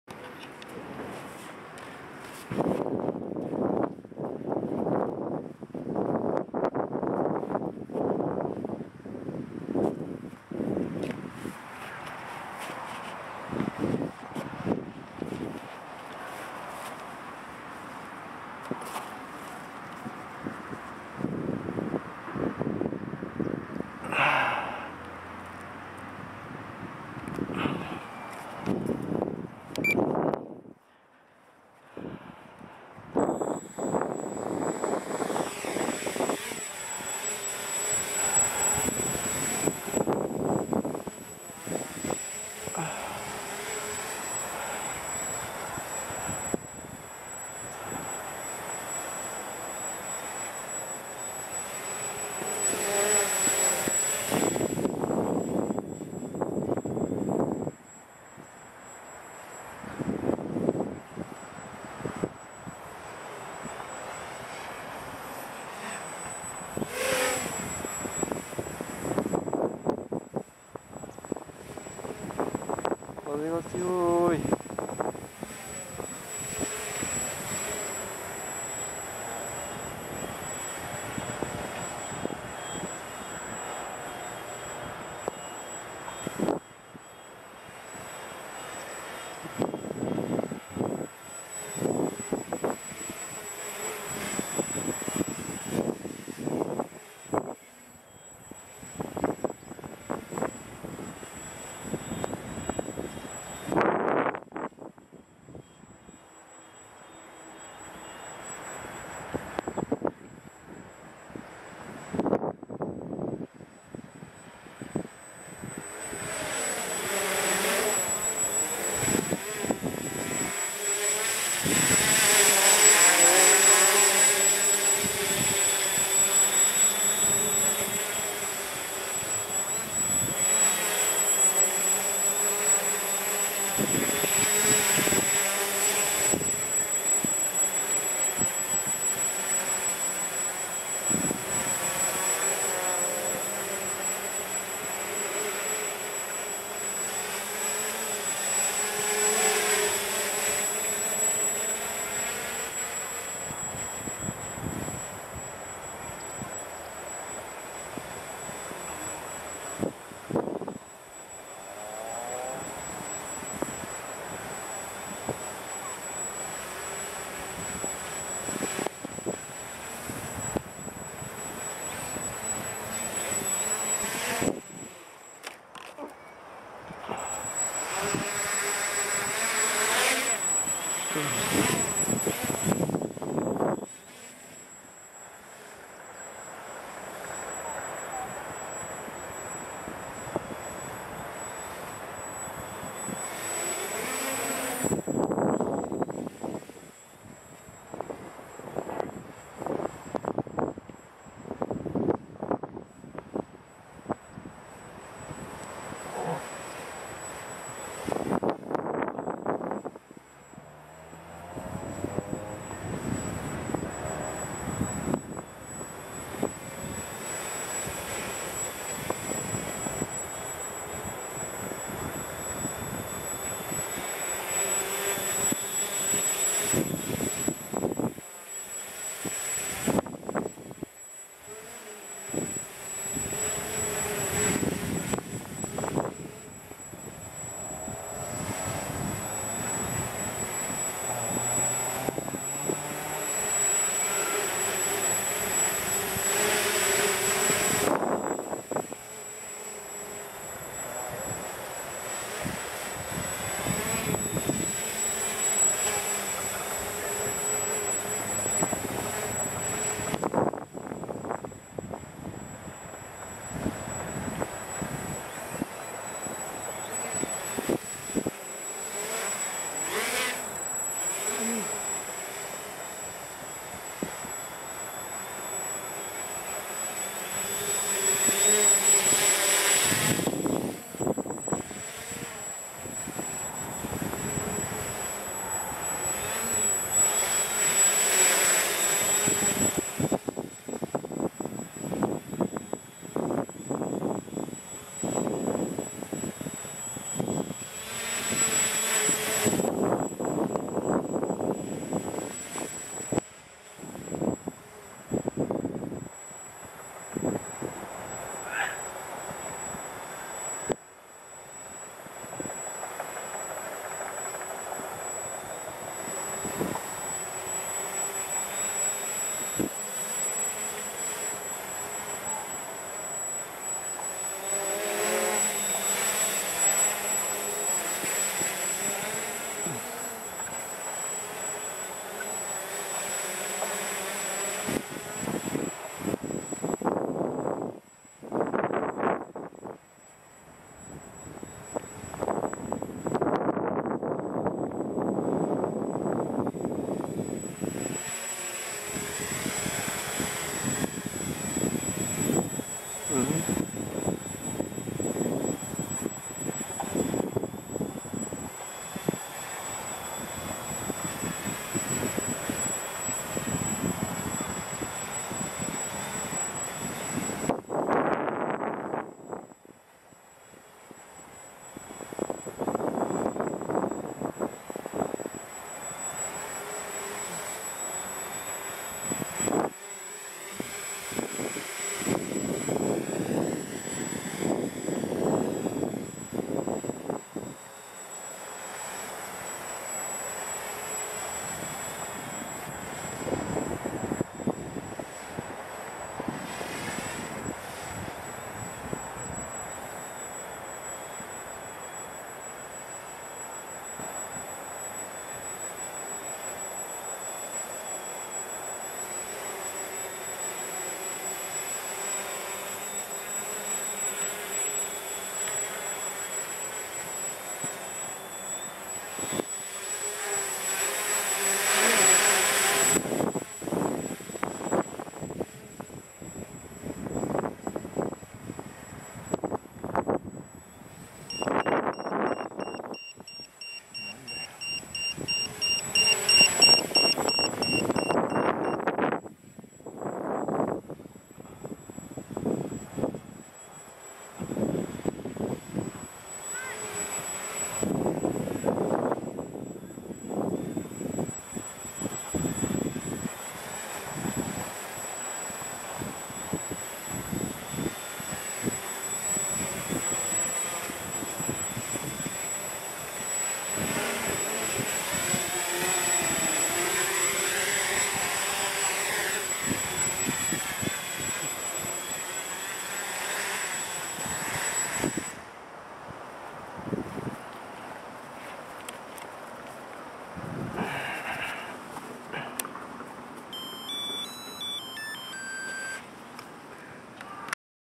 WALKERA Hoten-X DVR 808 #16 V2-Lens B デジカメ飛行動画18
フライトモードをノーマルから3Dモードに切り替えて
同時にデジカメで飛行動画も同時に録画しています
今日はMini DV DVR 808 #16 V2-Lens B をボデーの上に
載せて録画もしています、まだ未調整のためイマイチですが
ビックスワン、でのんびり飛んでます。